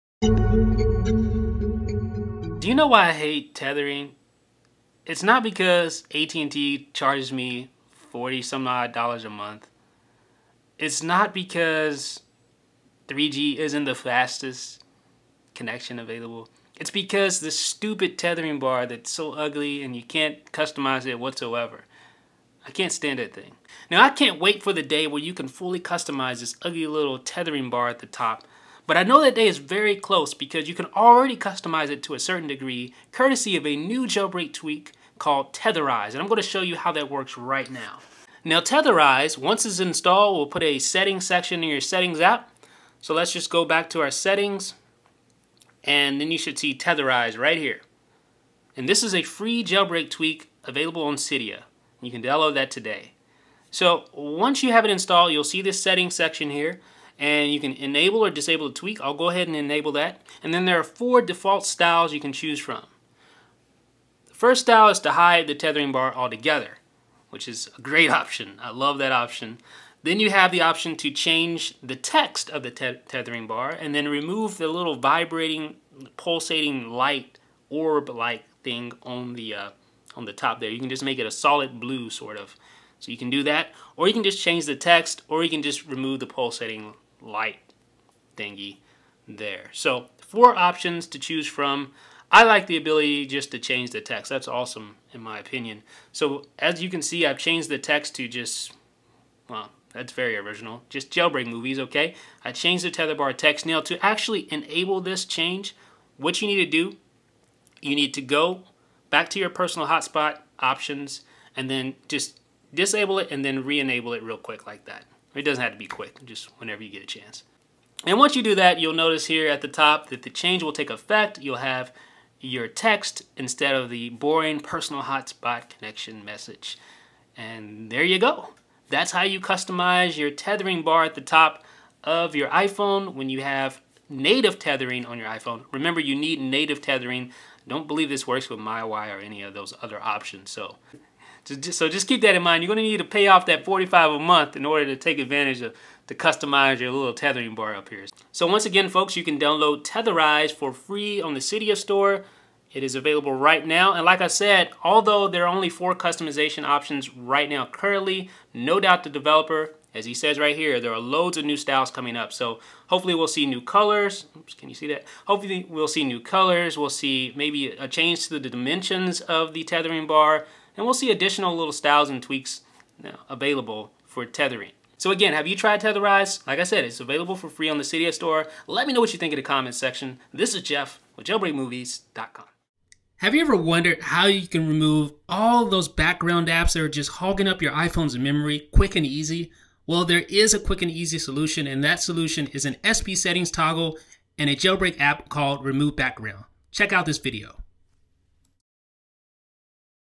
'Tetherize' Allows You to Customize Your iPhone's Native Tethering Bar
Want a cool way to customize your iPhone's boring old native tethering bar? Now you have options; introducing Tetherize, the first tether bar customization tweak for the iPhone.

About iDB